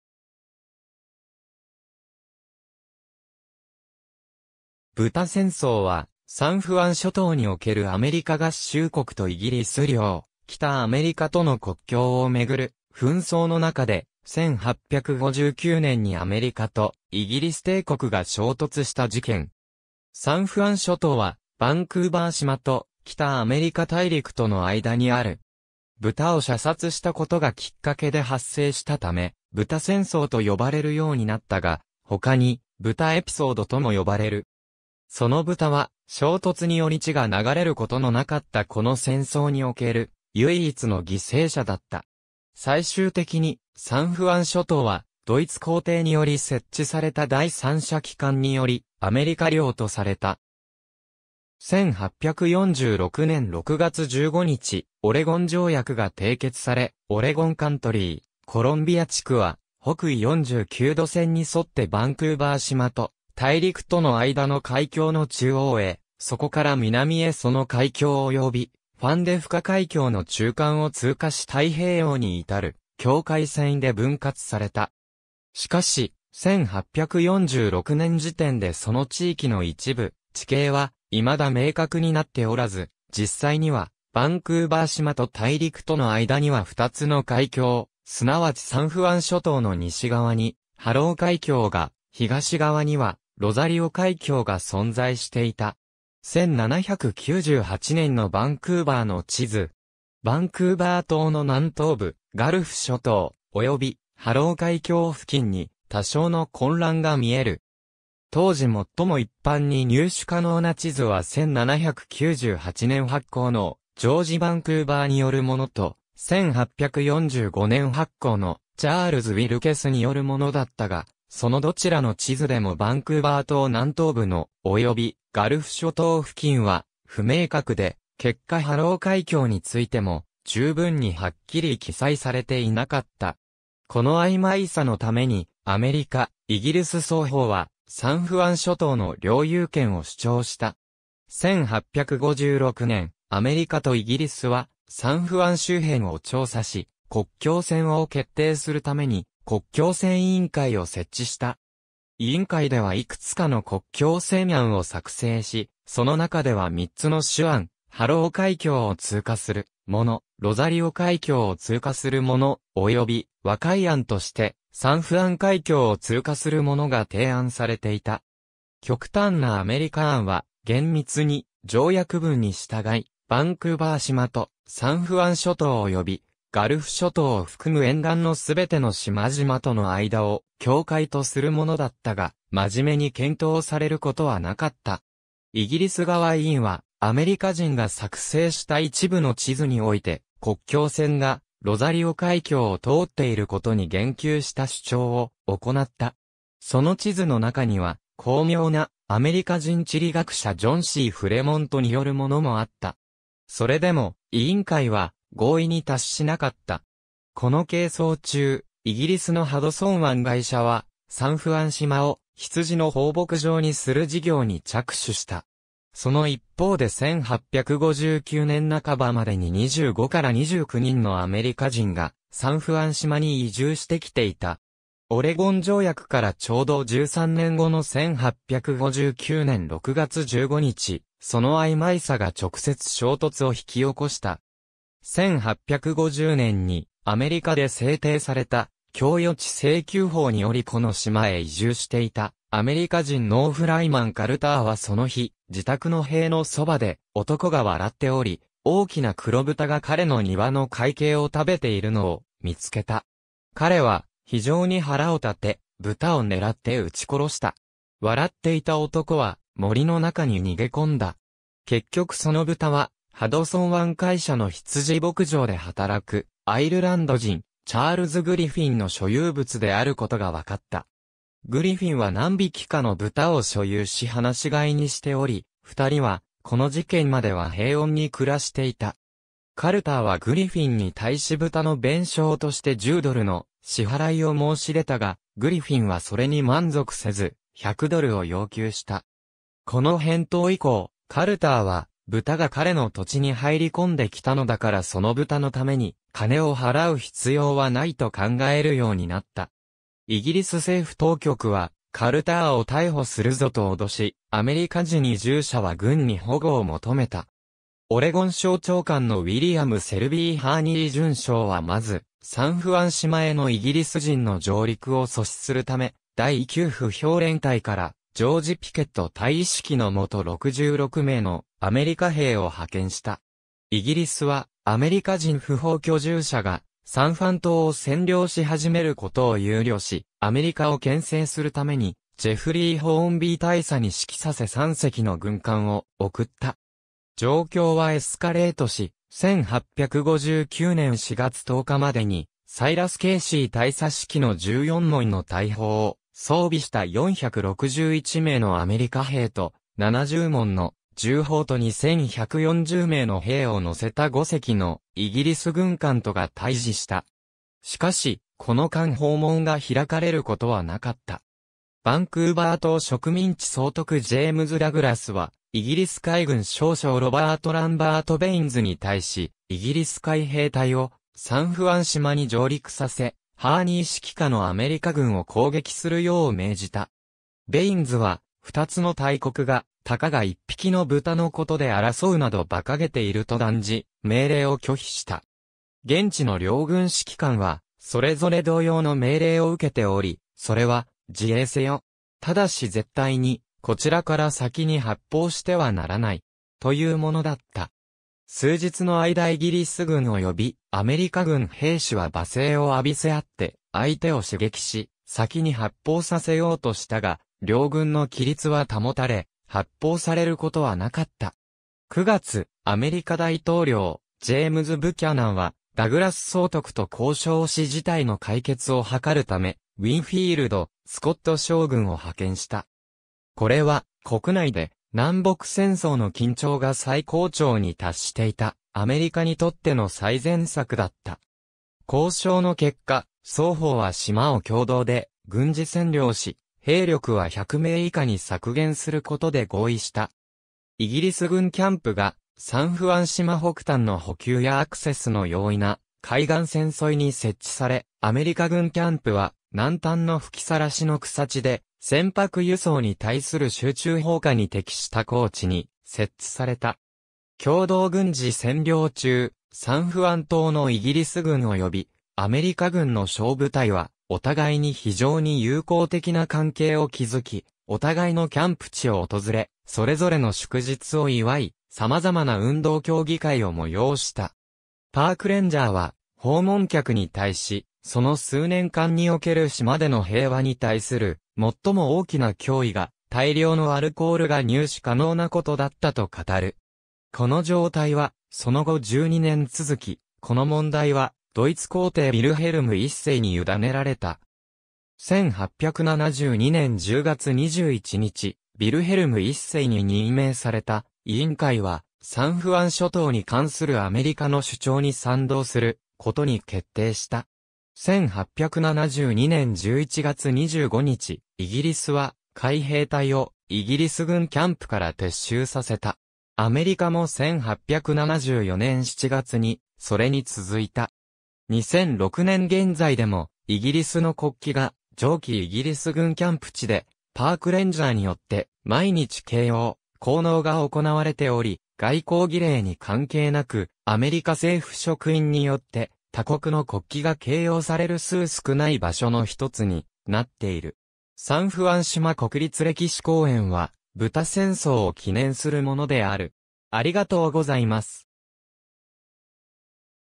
ブタ戦争 (サンフアン諸島)
死者が発生しなかった戦闘・紛争
ジェームズ・ブキャナン
ブタ戦争（ブタせんそう、英: Pig War）は、サンフアン諸島におけるアメリカ合衆国とイギリス領北アメリカ（後のカナダ）との国境をめぐる紛争（「サンフアン諸島国境紛争」あるいは「北西国境紛争」）の中で、1859年にアメリカとイギリス帝国が衝突した事件。
サンフアン諸島はバンクーバー島と北アメリカ大陸との間にある。
ブタを射殺したことがきっかけで発生したため「ブタ戦争」と呼ばれるようになったが、他に「ブタエピソード（Pig Episode）」とも呼ばれる。
そのブタは、衝突により血が流れることのなかったこの戦争における唯一の犠牲者だった。
最終的にサンフアン諸島はドイツ皇帝により設置された第三者機関によりアメリカ領とされた。
1846年6月15日、オレゴン条約が締結され、オレゴン・カントリー（アメリカ側の呼称）/コロンビア地区（イギリス側の呼称）は、北緯49度線に沿ってバンクーバー島と大陸との間の海峡の中央へ、そこから南へその海峡およびファンデフカ海峡の中間を通過し太平洋に至る境界線で分割された。
しかし、1846年時点でその地域の一部地形はいまだ明確になっておらず、実際にはバンクーバー島と大陸との間には2つの海峡、すなわちサンフアン諸島の西側にハロー海峡が、東側にはロザリオ海峡が存在していた。
1798年のバンクーバーの地図。
バンクーバー島の南東部、ガルフ諸島、およびハロー海峡付近に多少の混乱が見える。
当時もっとも一般に入手可能な地図は1798年発行のジョージ・バンクーバーによるものと、1845年発行のチャールズ・ウィルケスによるものだったが、そのどちらの地図でもバンクーバー島南東部のおよびガルフ諸島付近は不明確で、結果ハロー海峡についても十分にはっきり記載されていなかった。
このあいまいさのために、アメリカ、イギリス双方はサンフアン諸島の領有権を主張した。
1856年、アメリカとイギリスはサンフアン周辺を調査し、国境線を決定するために国境線委員会を設置した。
委員会ではいくつかの国境線案を作成し、その中では3つの主案、ハロー海峡を通過するもの、ロザリオ海峡を通過するもの、および和解案としてサンフアン海峡を通過するものが提案されていた。
極端なアメリカ案は“厳密に条約文に従い”、バンクーバー島と、サンフアン諸島およびガルフ諸島を含む沿岸の全ての島々との間を境界とするものだったが、真面目に検討されることはなかった。
イギリス側委員は、アメリカ人が作成した一部の地図において国境線がロザリオ海峡を通っていることに言及した主張を行った。
その地図の中には高名なアメリカ人地理学者ジョン・C・フレモントによるものもあった。
それでも委員会は合意に達しなかった。
この係争中、イギリスのハドソン湾会社はサンフアン島をヒツジの放牧場にする事業に着手した。
その一方で1859年半ばまでに25から29人のアメリカ人がサンフアン島に移住してきていた。
オレゴン条約からちょうど13年後の1859年6月15日、そのあいまいさが直接衝突を引き起こした。
1850年にアメリカで制定された供与地請求法（Donation Land Claim Act）によりこの島へ移住していたアメリカ人農夫ライマン・カルターはその日、自宅の塀のそばで男が笑っており、大きな黒ブタが彼の庭の塊茎を食べているのを見つけた。
彼は非常に腹を立て、ブタを狙って撃ち殺した。
笑っていた男は森の中に逃げ込んだ。
結局そのブタは、ハドソン湾会社のヒツジ牧場で働くアイルランド人チャールズ・グリフィンの所有物であることが分かった。
グリフィンは何匹かのブタを所有し放し飼いにしており、二人はこの事件までは平穏に暮らしていた。
カルターはグリフィンに対しブタの弁償として10ドルの支払いを申し出たが、グリフィンはそれに満足せず、100ドルを要求した。
この返答以降、カルターはブタが彼の土地に入り込んで来たのだからそのブタのために金を払う必要はないと考えるようになった（信憑性の低い話ではあるが、カルターはグリフィンに対し「そいつはオレのジャガイモを食ってたんだよ」と言い、グリフィンは「おまえのジャガイモがオレのブタに食われないようにするのはおまえの責任だろ」と返したという）。
イギリス政府当局はカルターを逮捕...